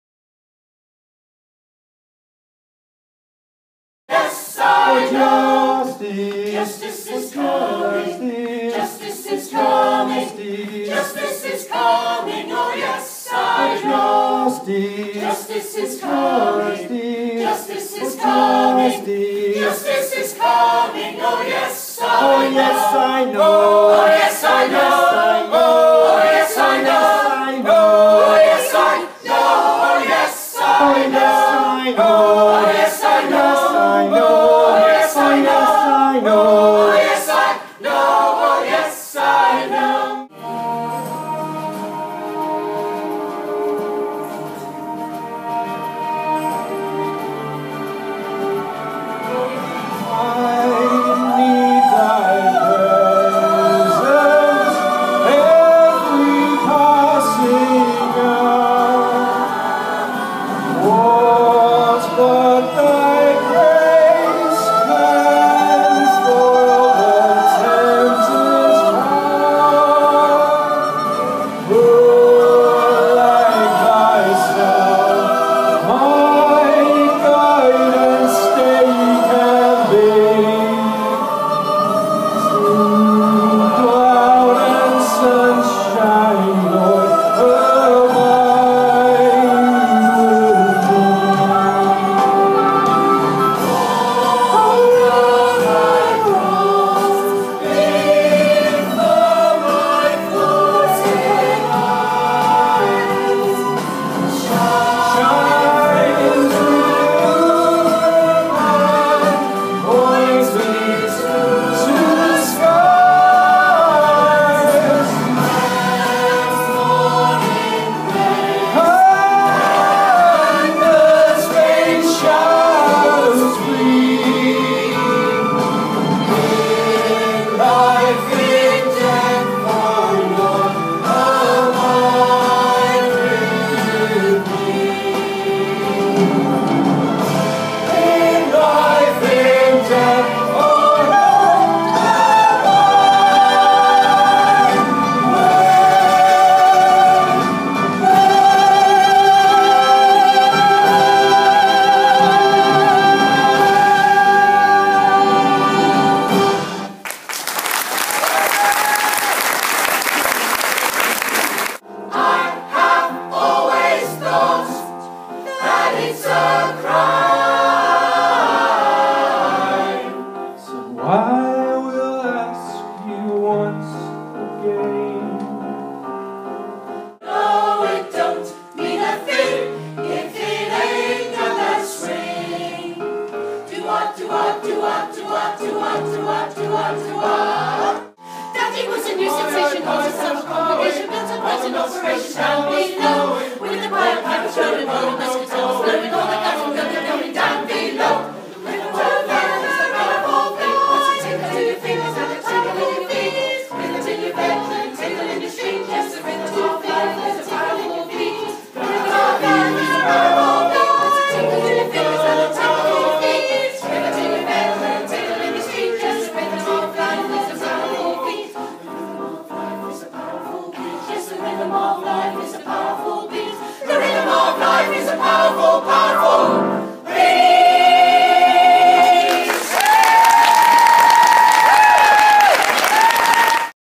The Royal Mail Choir Do Chipping Sodbury
Two thirds of the Royal Mail Choir perform at the Chipping Sodbury Town Hall on the 28th June 2013.